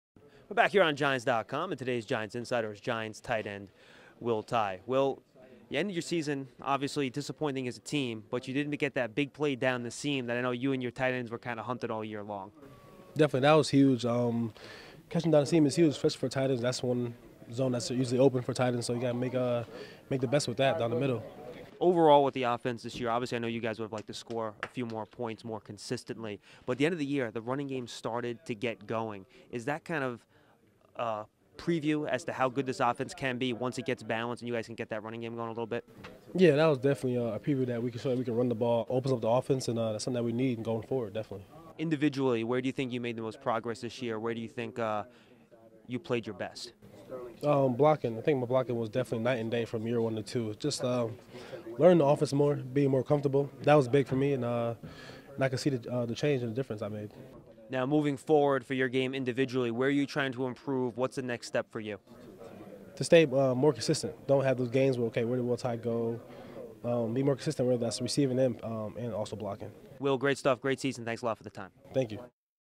Giants Insider: TE Will Tye
Will Tye chats with about his 2016 season and offseason goals